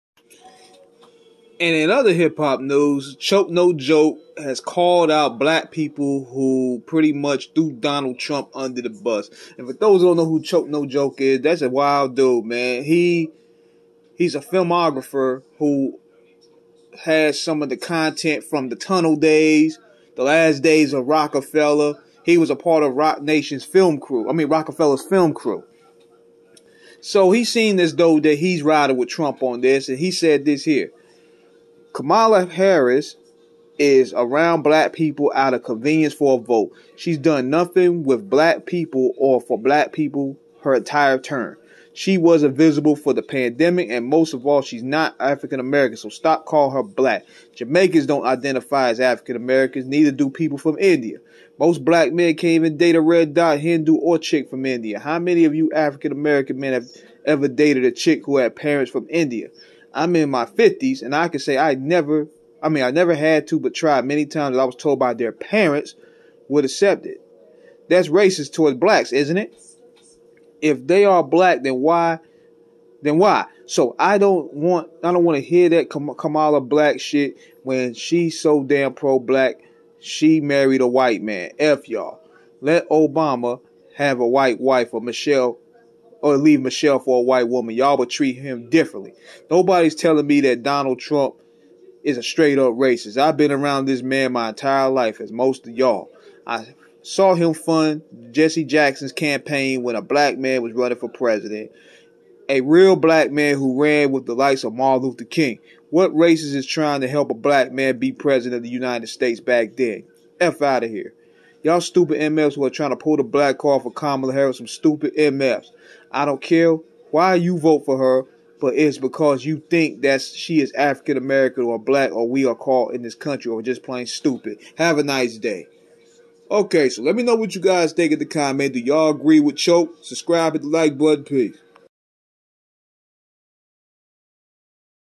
Choke No Joke CALLS OUT Black People whom abandoned Donald Trump
Yall Agree?